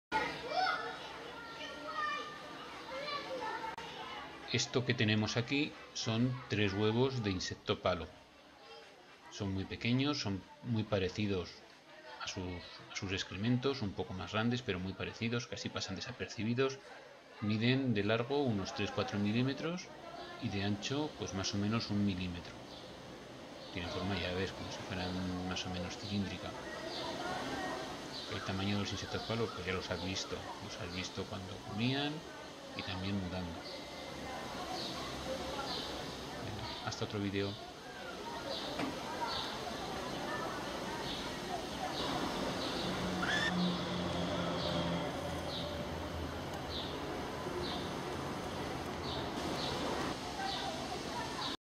Huevos de insecto palo (Sipyloidea sipylus)
Aquí puedes ver cómo son los huevos de los insecto palo (Sipyloidea sipilus).